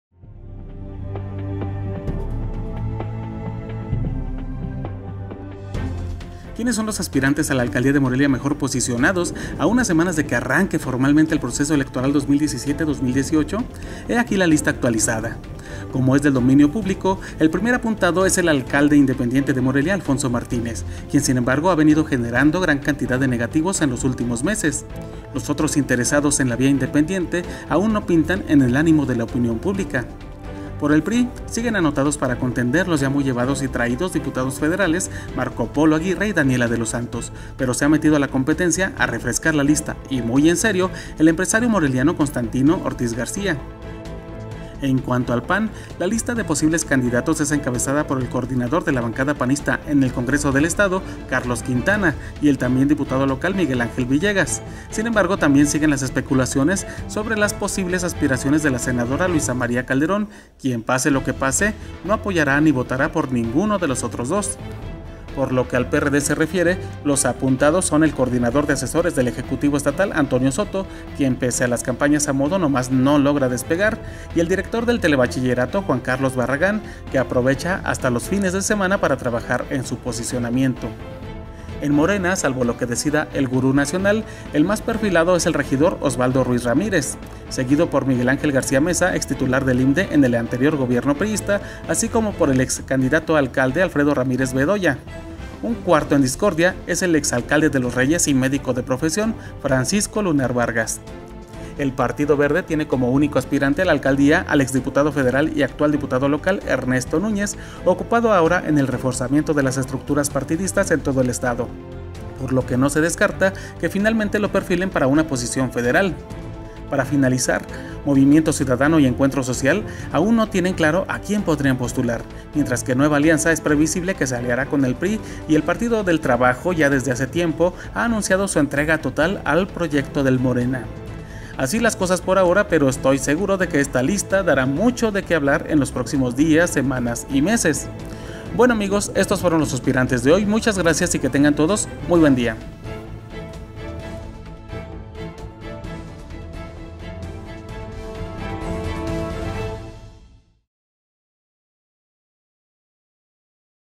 Los suspirantes... ¿Quiénes por la alcaldía de Morelia? (Por: Nicolás Casimiro)
¿Quiénes son los aspirantes a la alcaldía de Morelia mejor posicionados a unas semanas de que arranque formalmente el proceso electoral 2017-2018? He aquí la lista actualizada.